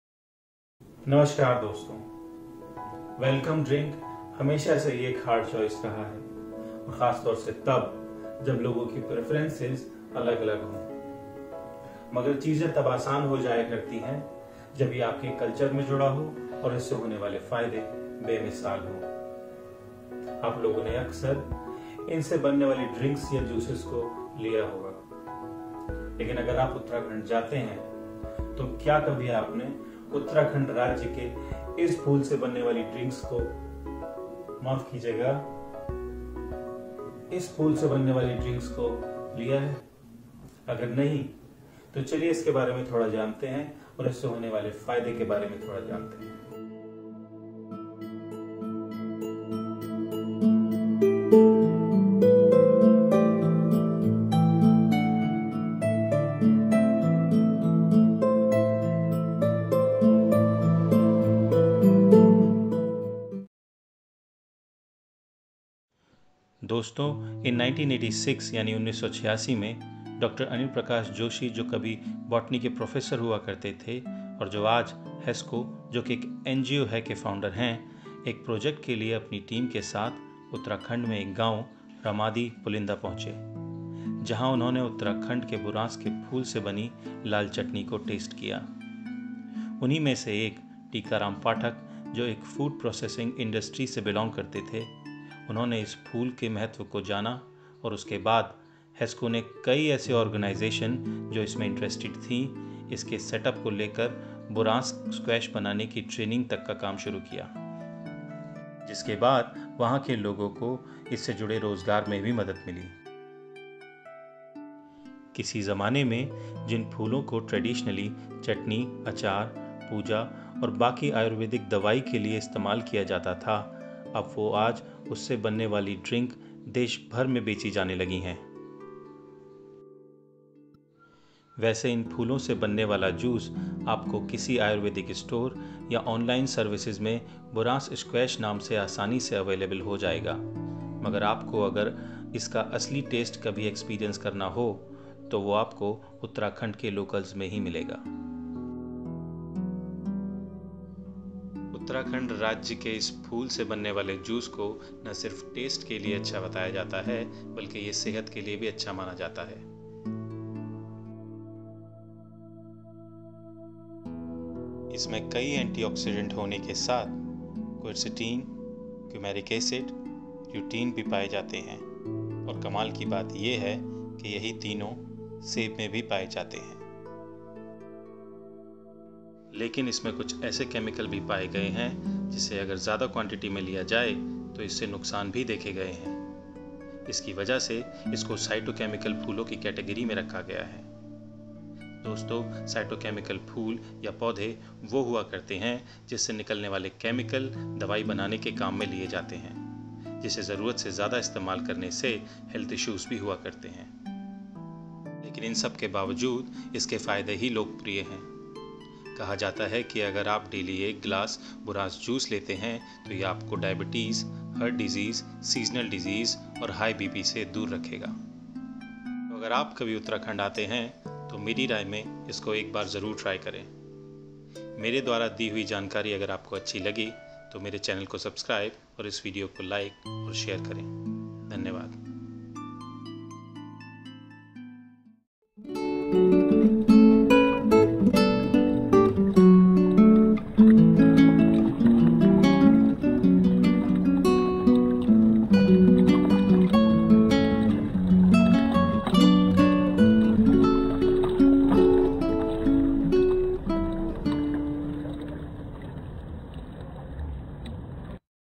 Buransh (Rhododendron arboretum ) | Uttrakhand's state tree | A God's Land
Rhododendron arboretum is it's scientific name and Buransh tree is one of the symbol of Uttrakhand state.

It has six species in Uttrakhand.

Information in this video has been retrieved from different sources.